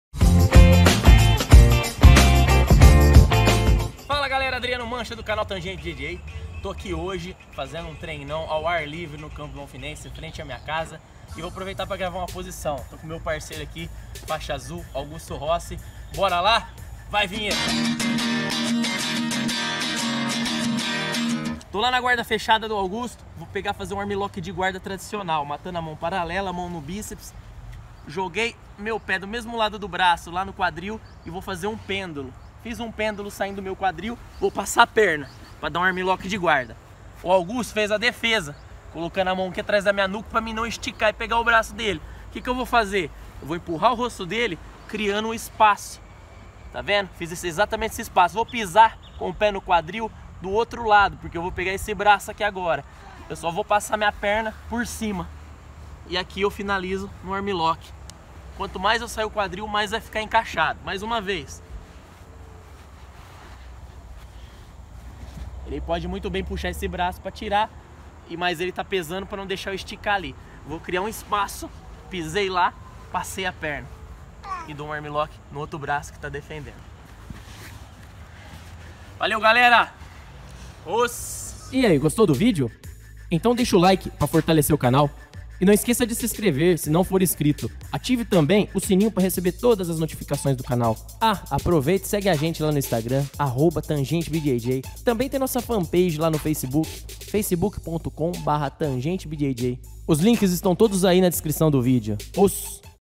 FINALIZAÇÃO - armlok no braço oposto na defesa do armlok - ADRIANO MANCHA - JIU JITSU
Fala galera!!
Se liga nessa finalização que gravei com meu brother Augusto Rossi para vcs aqui do Canal! Espero que gostem, OSS.